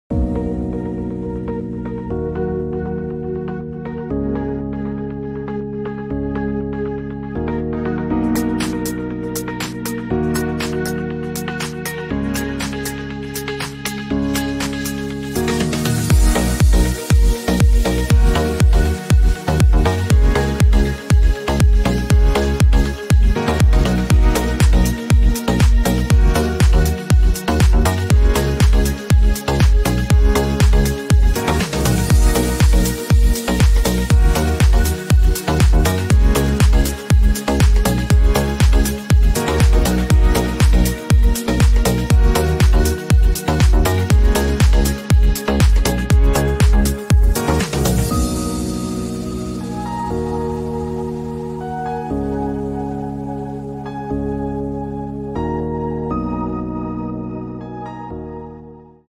1 Sprite Way, BRASSALL, QLD, 4305
🔔 For Sale - BRAND NEW IN BRASSALL! FIRST HOME BUYERS DON'T MISS OUT ON 30K IN GOVERNMENT GRANTS ENDS 31ST MARCH!

For all enquiries contact Dylan Ansems